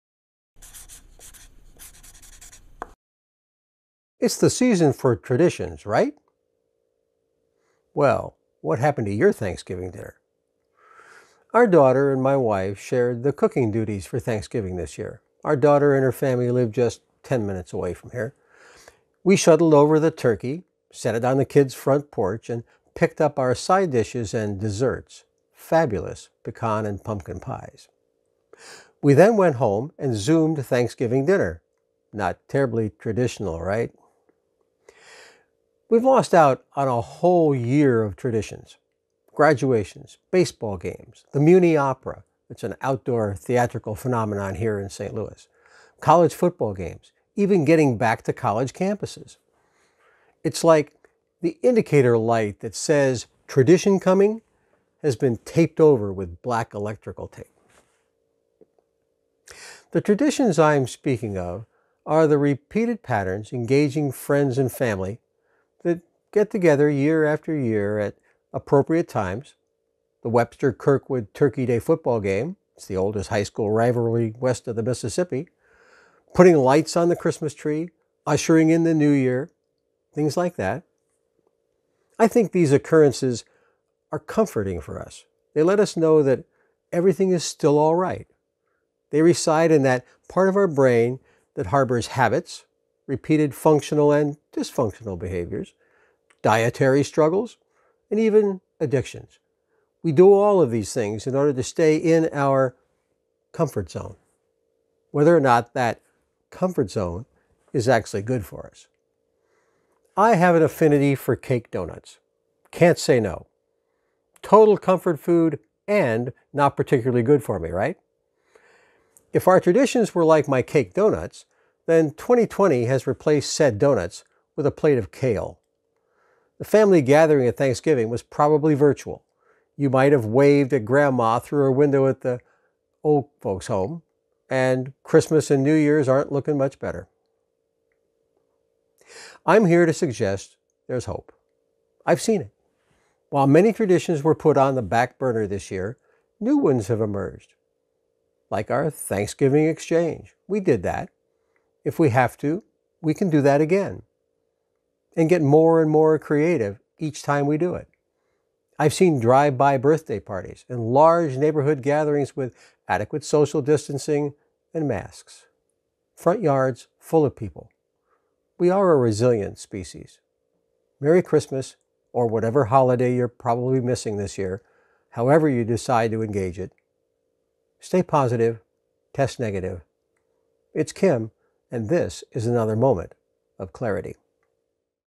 Moment of Clarity, December 2020...loss of traditions and our resilience,
Many of our social traditions have been canceled in 2020.  Many such gatherings have morphed into virtual or social distanced versions.  Many have been re-scheduled.  Many have simply been re-created within the boundaries we're compelled to operate under.